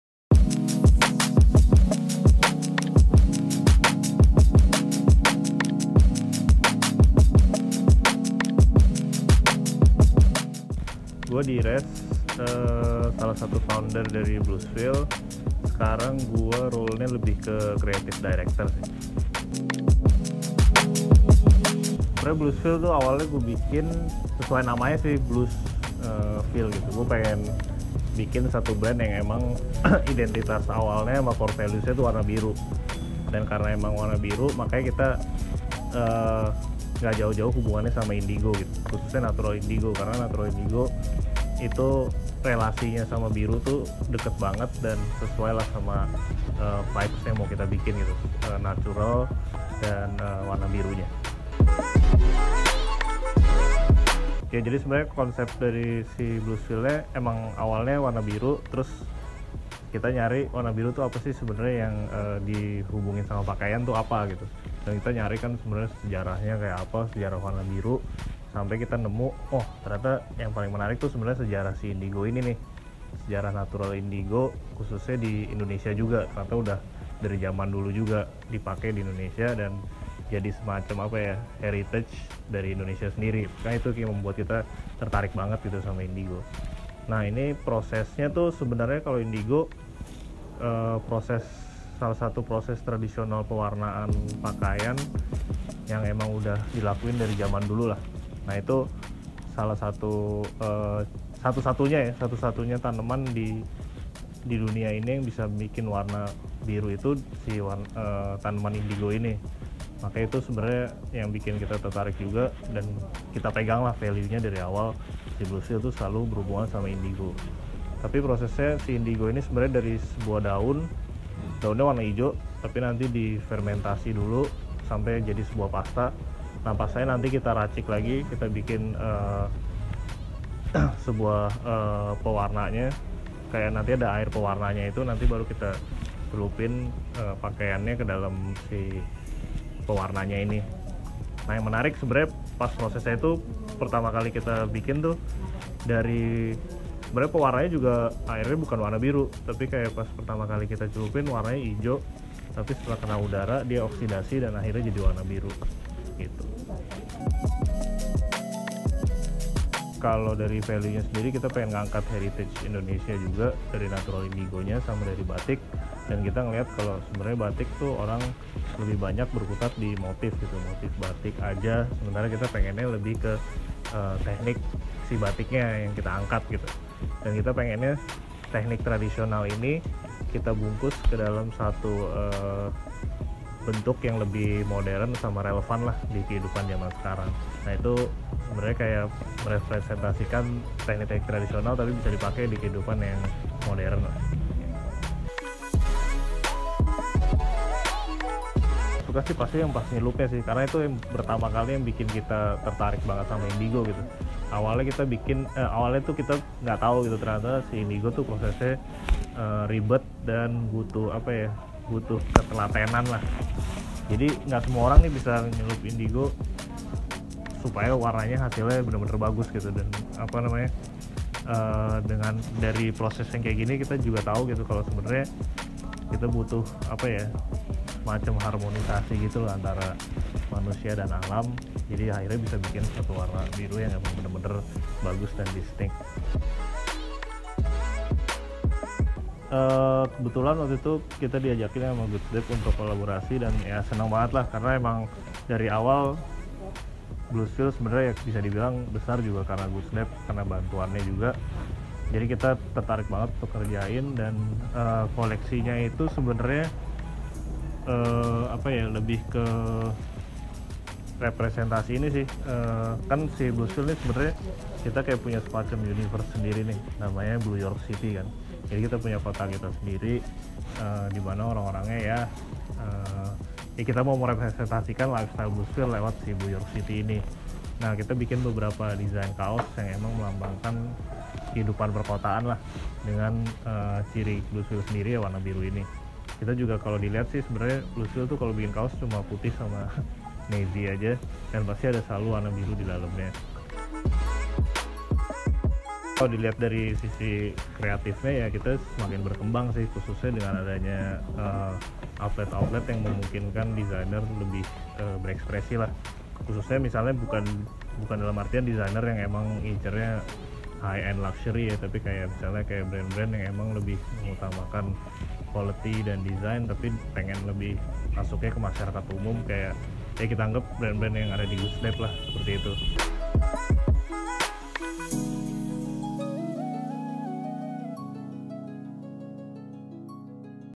Behind The Brand: Bluesville
We go behind the brand of Bluesville. Direz (co-founder) tell us about how they started the brand and how is the process of natural coloring with indigo.

Videographer: Arzanda
Editor: Arzanda